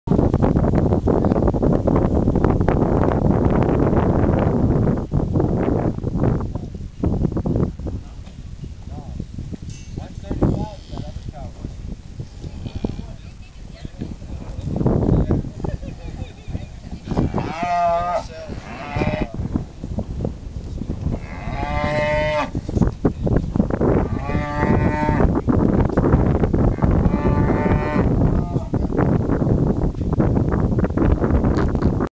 Branding 2009 001
Cattle branding 2009 on the Navajo Reservation.